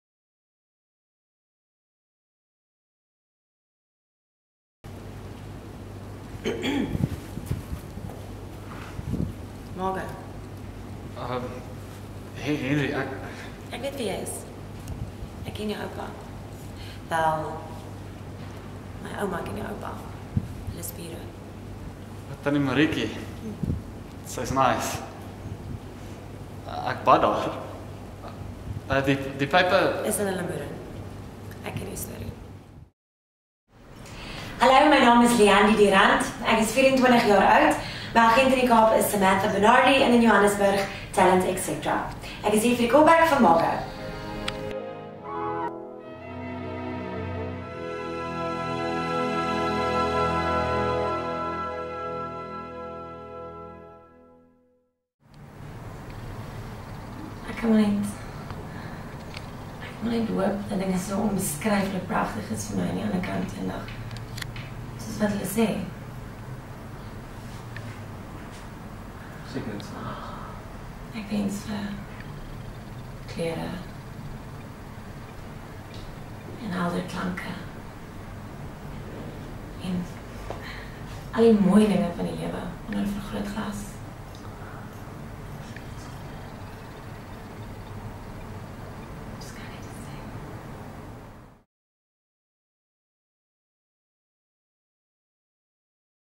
The search for the leading lady
The best thing to happen to someone is being told that they will be in a movie! Leandi du Randt will play the role of Margo in the film Die Windpomp! Frankie du Toit, should be a talent scout!